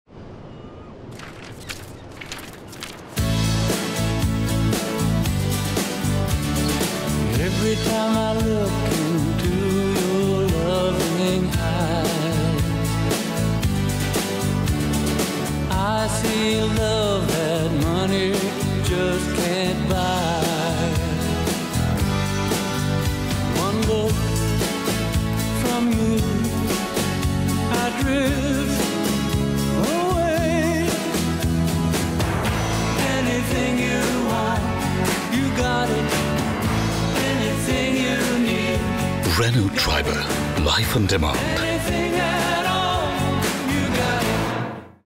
Renault Triber | life on demand
Renault Triber’s four modes & endless possibilities accommodate life on demand. Presenting our brand new TVC featuring the amazing Amit Sadh whose passion for drives with his tribe is second to none.